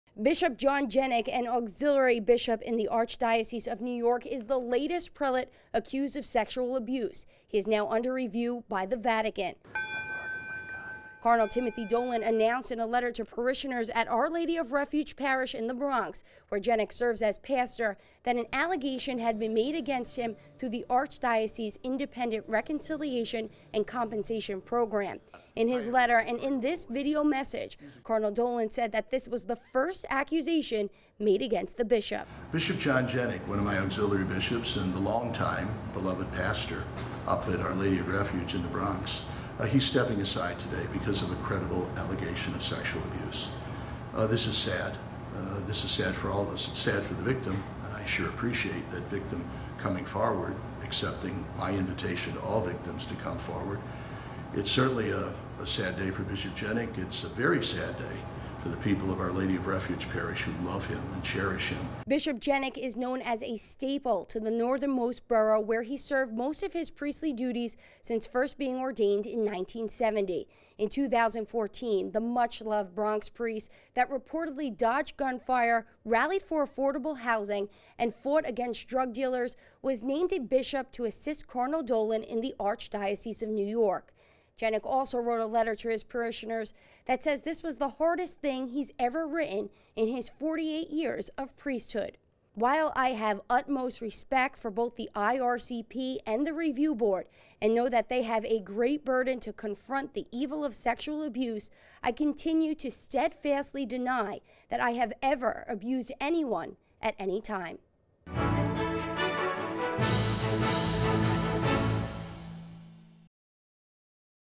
New York Auxiliary Bishop Under Review
In a letter dated October 29, sent to parishioners at Our Lady of Refuge parish in the Bronx, Cardinal Timothy Dolan announced that an allegation had been made against Auxiliary Bishop Jenik through the archdiocese’s Independent Reconciliation and Compensation Program (IRCP).